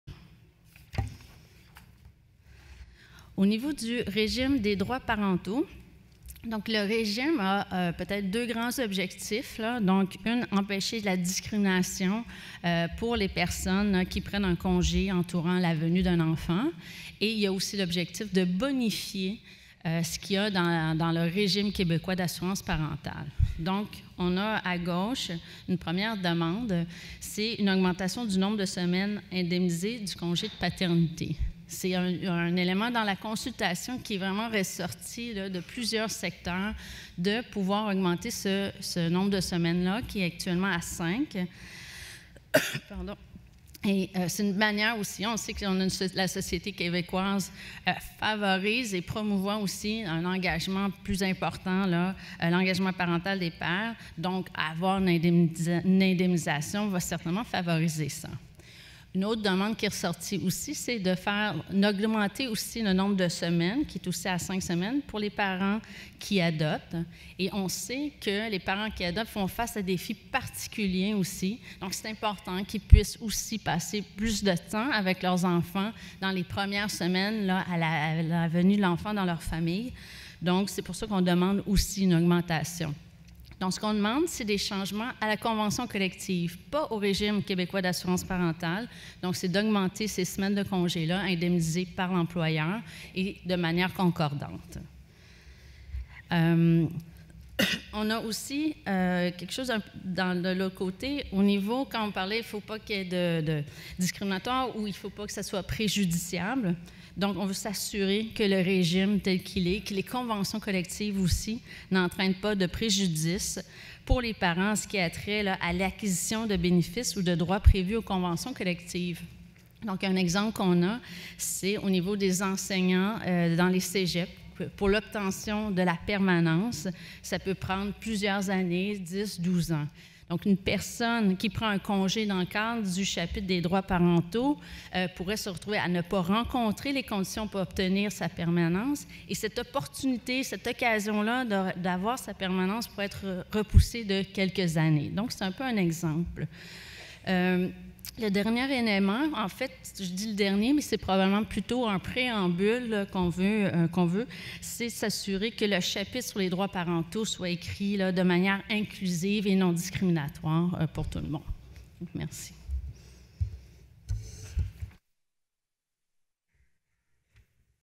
Présentation des demandes de table centrale - Partie 7 - Droits parentaux
Partie 7 de la présentation : Droits parentaux
Présentation par Julie Audet, conseillère syndicale à la négociation pour le CCSPP

Le 30 octobre dernier, la CSN a déposé ses demandes de table centrale au Secrétariat du Conseil du trésor. Le 29 octobre, le Comité de coordination des secteurs public et parapublic (CCSPP) faisait la présentation complète de ces demandes aux syndicats. Cette présentation est dirigée par Frédéric Lavigne, coordonnateur du CCSPP, Mathieu St-Onge, conseiller syndical à la négociation pour le CCSPP et Julie Audet, conseillère syndicale à la négociation pour le CCSPP.

Date de la présentation : 29 octobre 2019
Publication originale : 29 novembre 2019